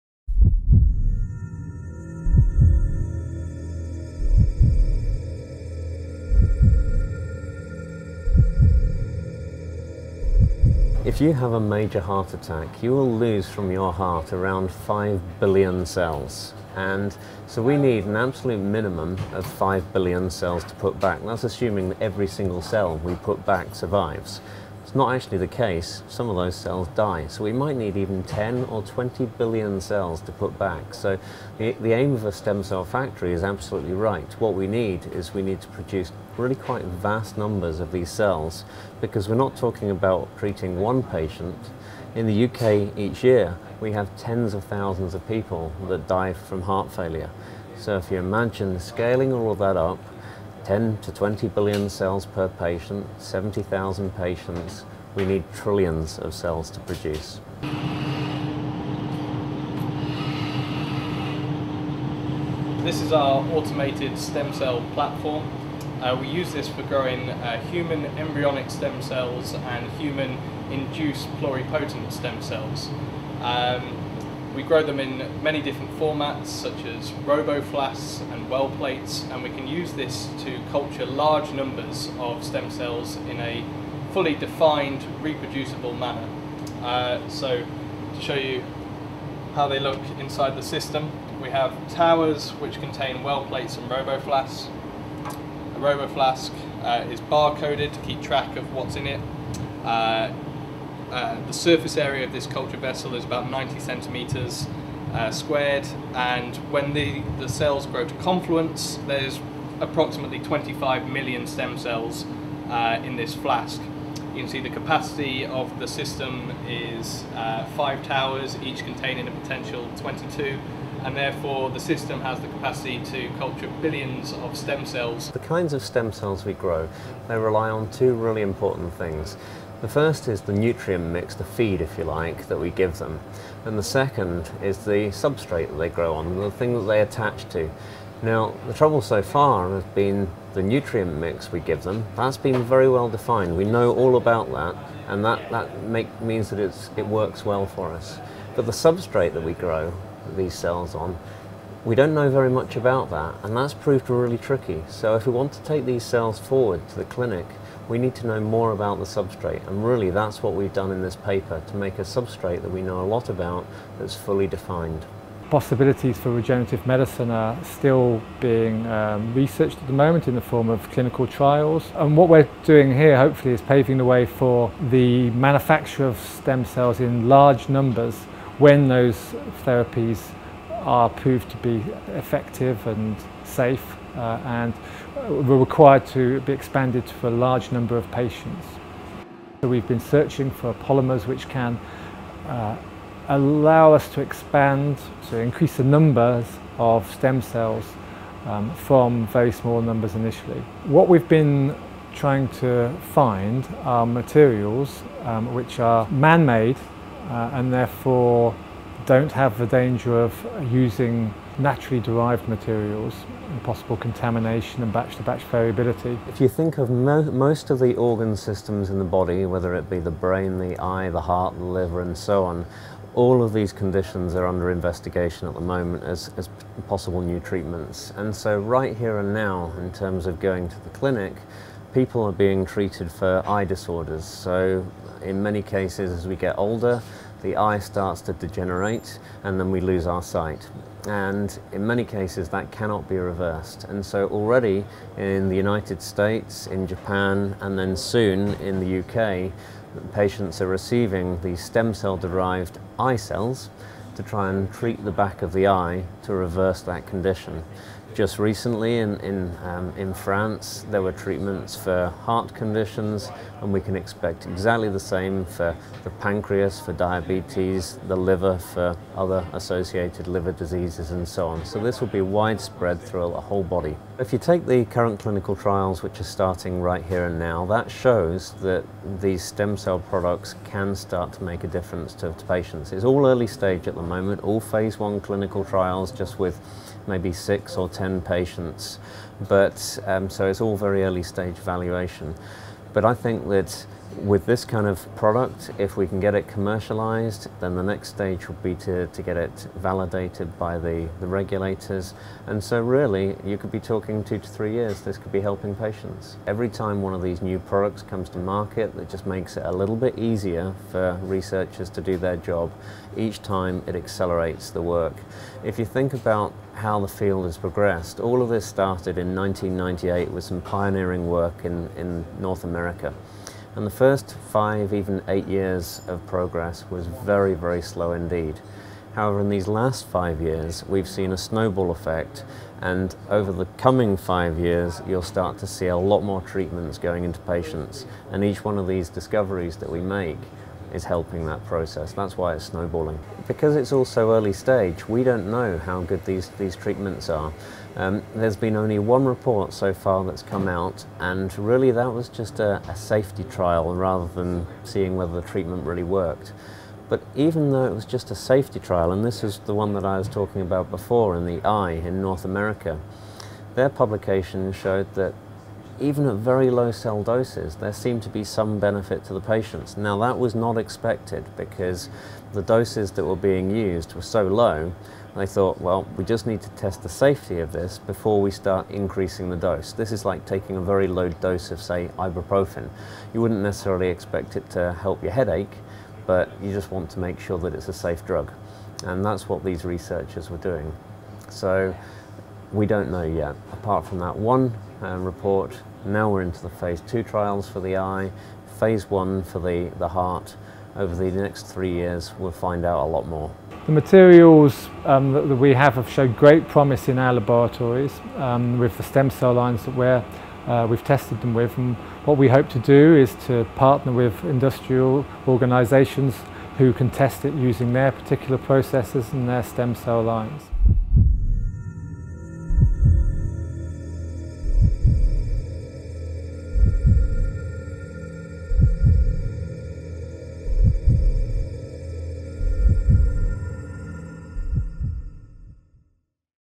New material forges the way for ‘stem cell factories’ of the future
Experts at The University of Nottingham have discovered the first fully synthetic material with potential to grow billions of stem cells. The discovery could forge the way for the creation of 'stem cell factories' - the mass production of human pluripotent stem cells - for clinical use in the treatment of the heart, liver and brain.